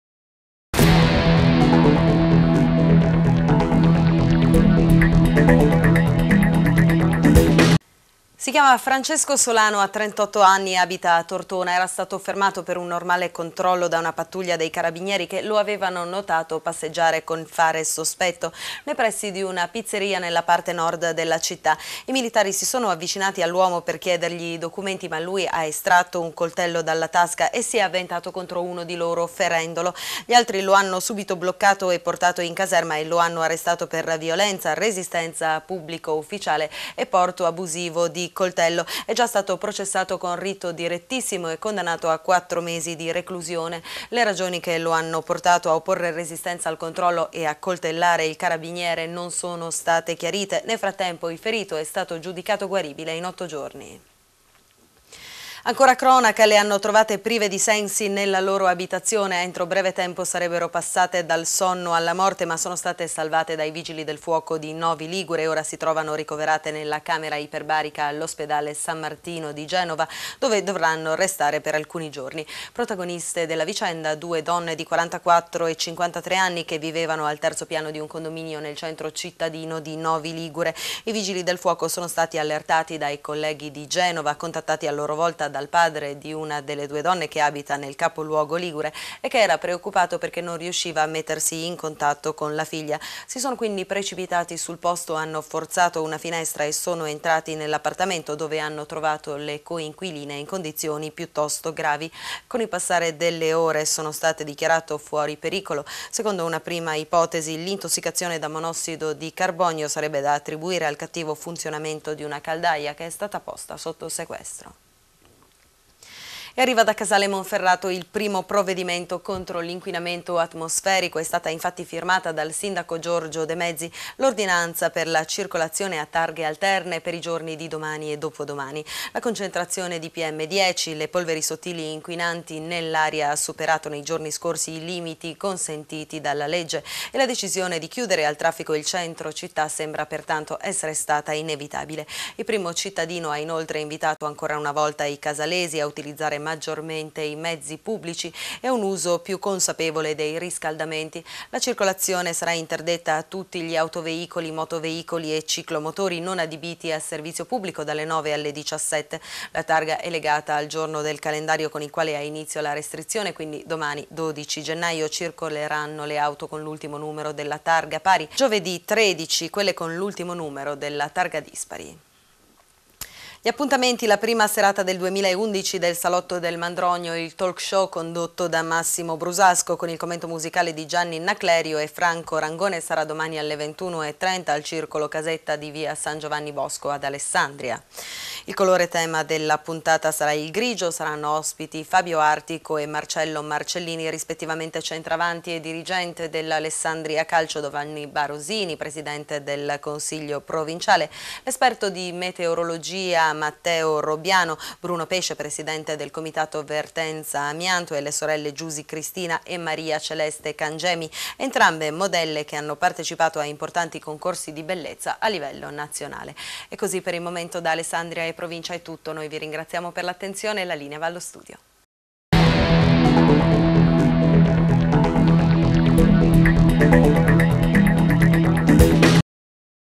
Notizie del Giorno Alessandria 11 Gennaio
Tortona-  Francesco Solano,è stato fermato per resistenza a pubblico ufficiale
Novi Ligure - Due donne trovate nella loro abitazione prive dei sensi causa esalazzione monossido di carbonio.
Casale Monferrato - Domani e Giovedì si circola a targhe alterna causa alzamento delle pollveri sottili.
Alessandria la prima serata del"Salotto delMandrogno" saràdedicata ai Grigi